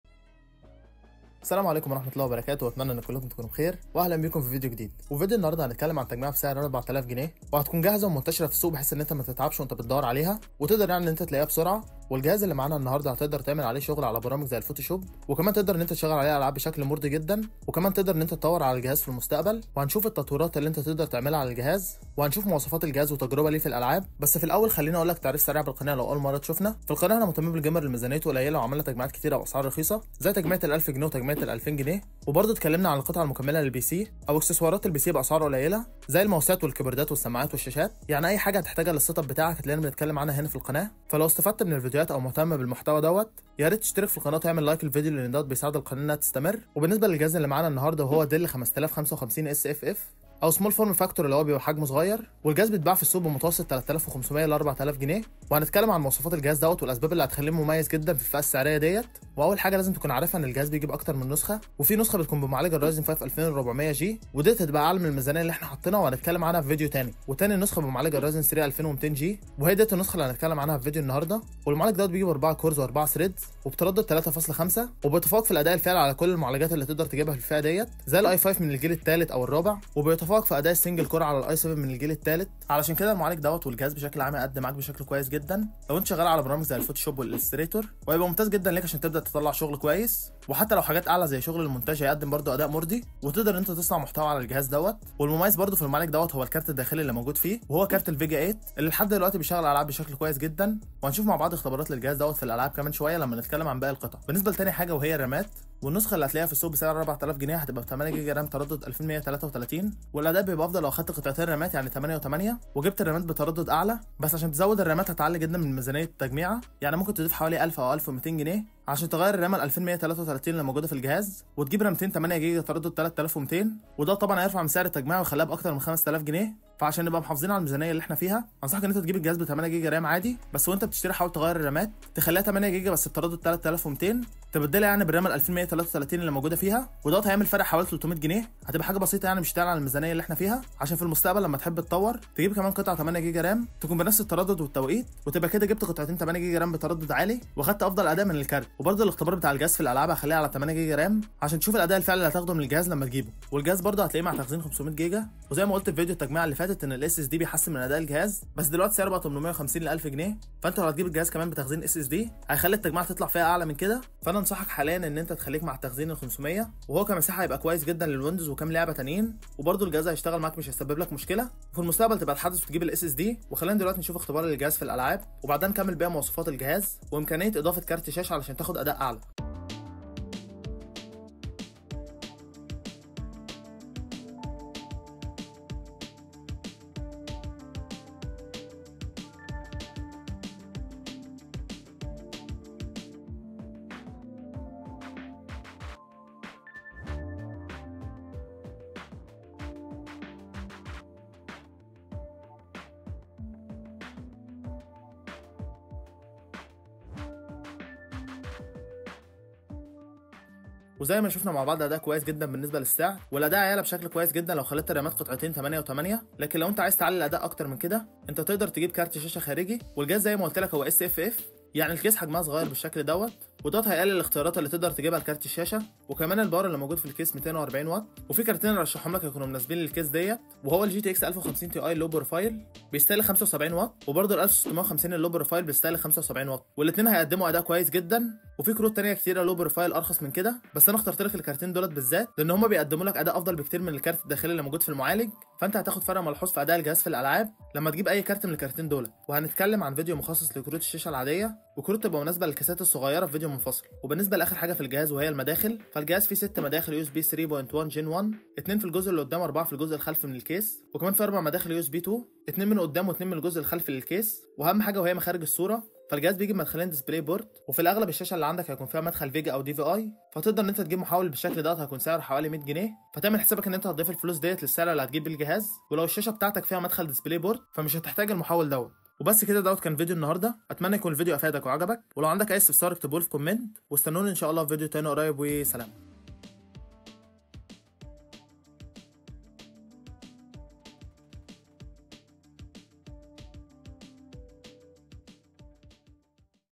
تجميعة كمبيوتر ب 4000 جنية بمعالج ryzen 3 2200g افضل تجميعة pc في 2024
فيديو النهارده هنتكلم عن افضل تجميعة ب 4000 جنيه للالعاب مع معالج ryzen 3 2200g   تقدر تشغل عليها العاب في سنة 2024 وهتكون تجميعه جاهزه بعد غلاء الاسعار وهتكون افضل تجميعة ب 4000 جنية في 2024  تقدر تجيبها  و تقدملك اداء مميز جدا بالنسبه لسعرها